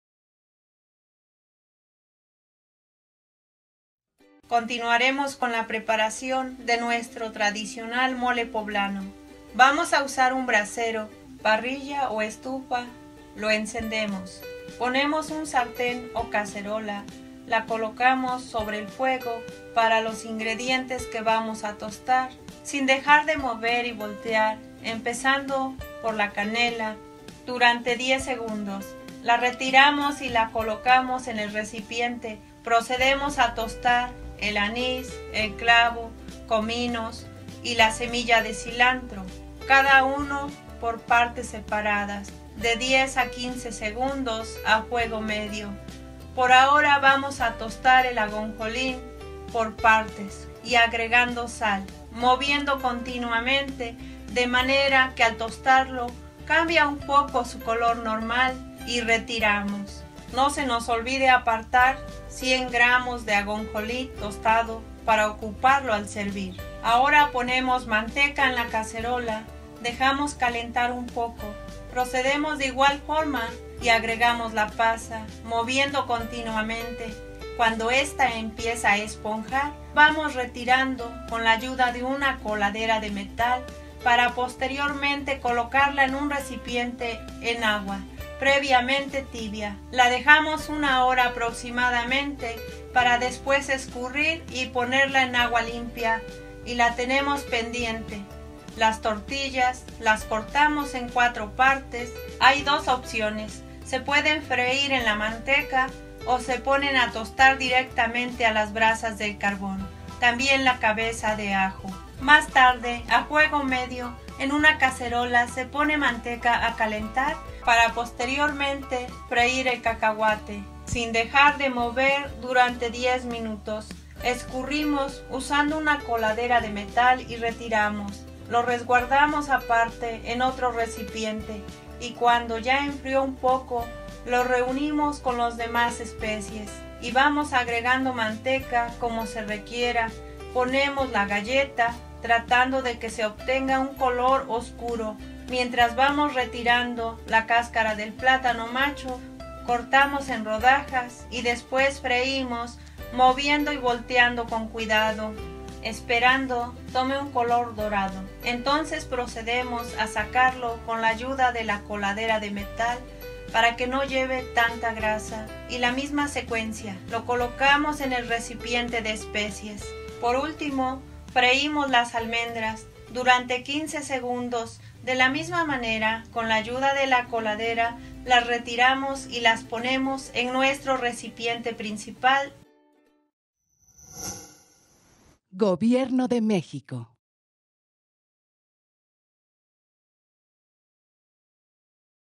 ComunidadContigo: El Mole Poblano, 4/5
DESCRIPCIÓN DETALLADA DE LA ELABORACIÓN ARTESANAL DEL TRADICIONAL MOLE POBLANO QUE SURGE A PARTIR DE REUNIR EL CONOCIMIENTO CULINARIO DE MUJERES (EN SU MAYORÍA LONGEVAS) DE LA COMUNIDAD DE SAN NICOLÁS ZOYAPETLAYOCA EN TEPEACA, PUEBLA. QUIENES A TRAVÉS DEL TIEMPO PRESERVAN ESTE DELICIOSO PLATILLO.